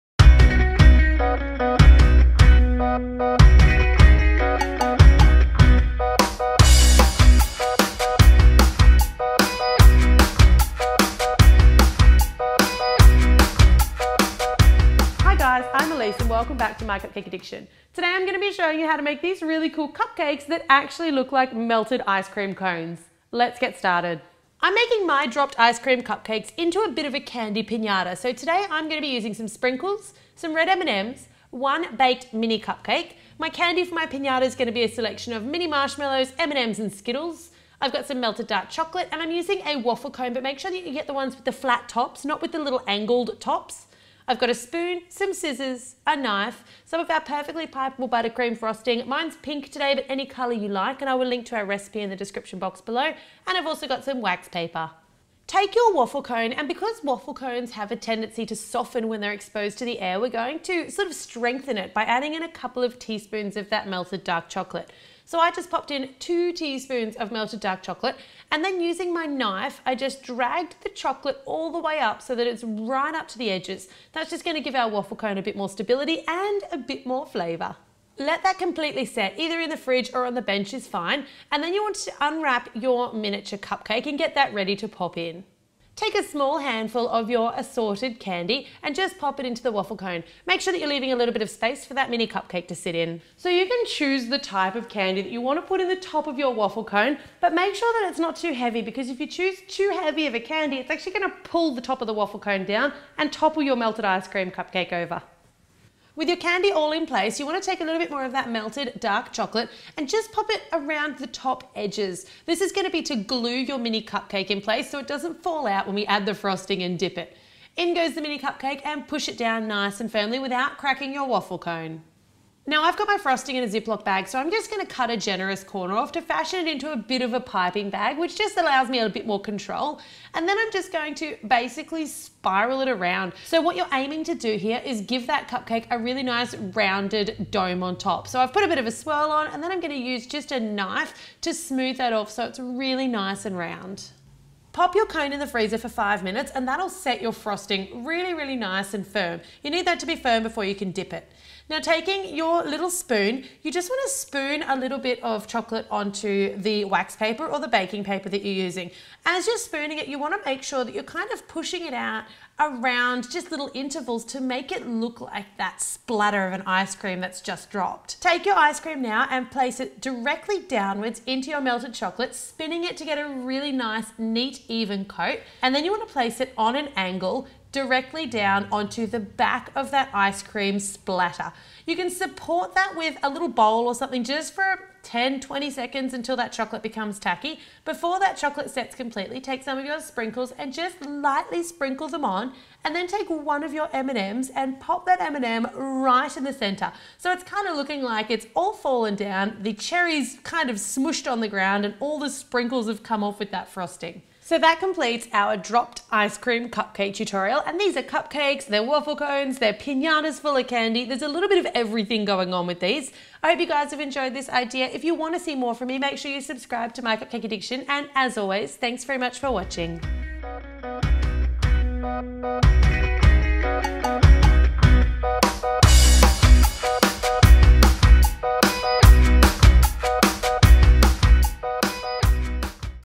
Dropped Ice Cream Pinata Cupcakes by Cupcake Addiction
Today I'm literally dropping a cupcake on it's head! I show you how to make an inverted cupcake filled with candy and designed to look like a melted ice cream!

Mini cupcakes, baked (no wrappers required)
Candy (optional) make sure its not to heavy
Waffle cones (make sure its got the flat rim on top)
Sprinkles
Red m&m's

Tools:
Knife
Spoon
Scissors
Parchment Paper
Snap Seal Bags

I'd love to see pics of your cakes and cake pops on ANY of the above -- You all know how mine turn out -- now show me YOUR creations!

Xx Elise